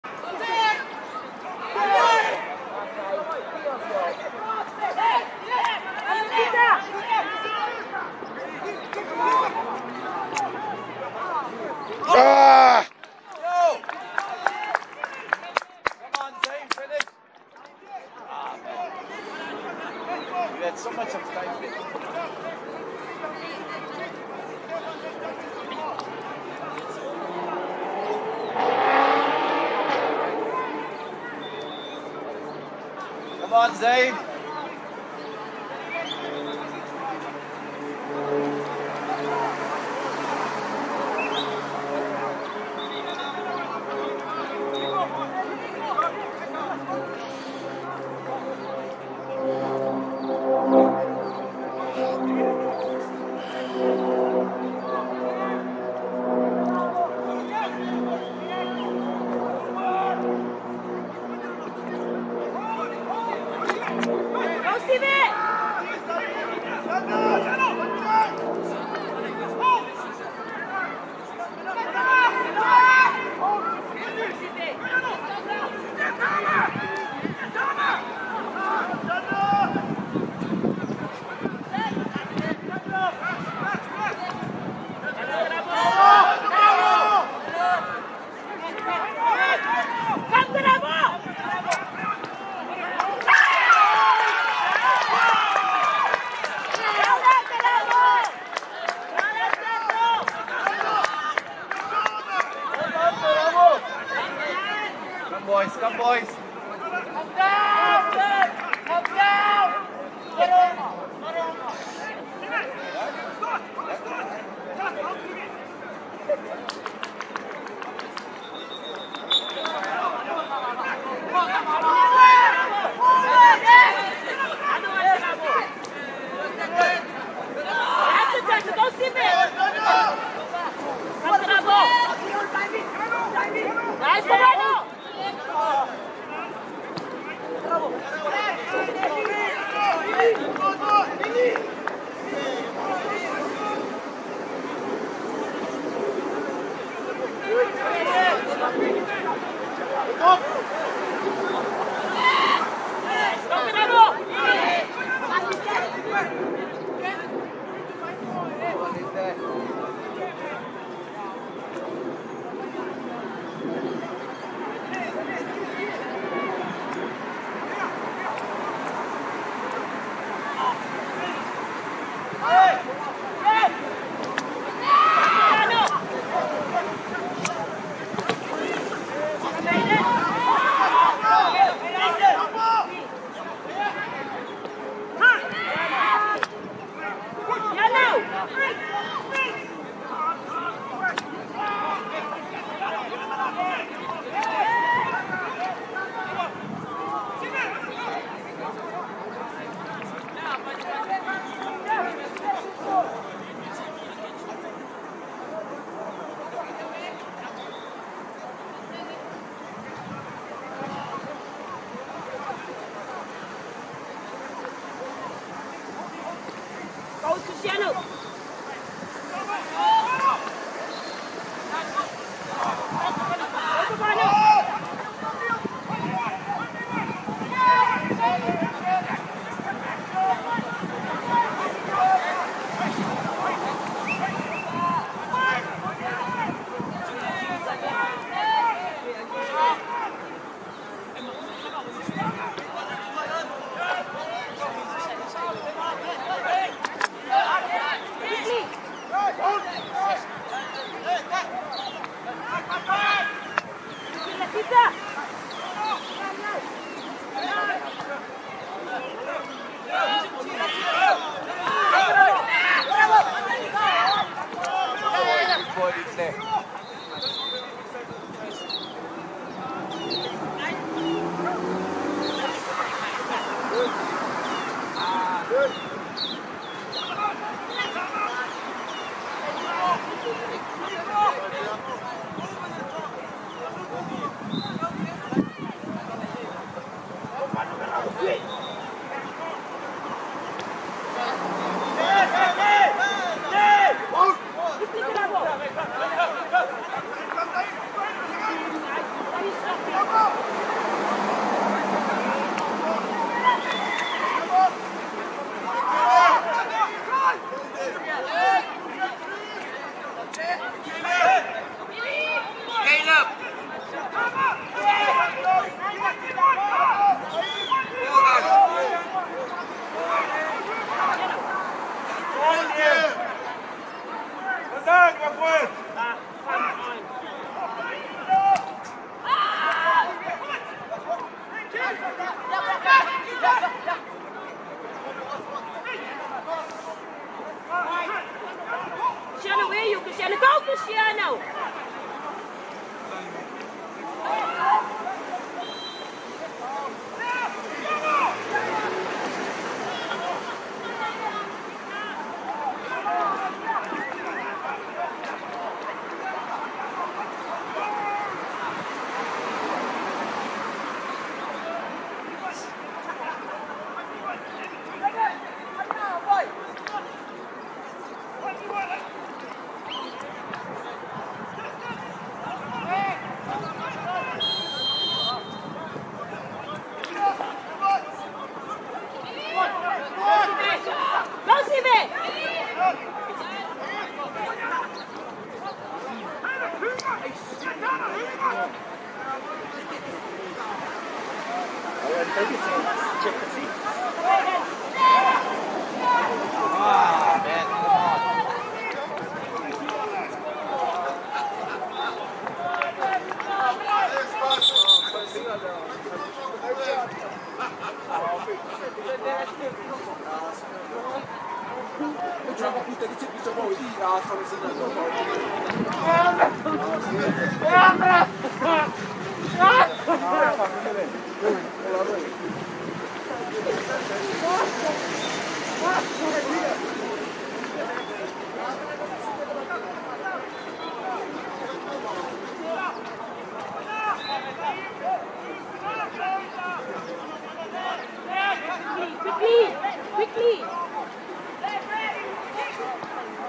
Supersport United u15 vs Orlando Pirates u15 part2
Part 2 of the highlights of the Supersport United FC u15 vs Orlando Pirates game, played on the 24th of April 2021